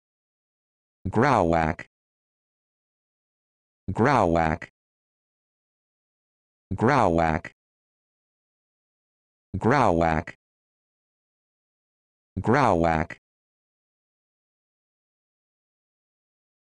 How to Pronounce Grauwacke
This video shows you how to pronounce Grauwacke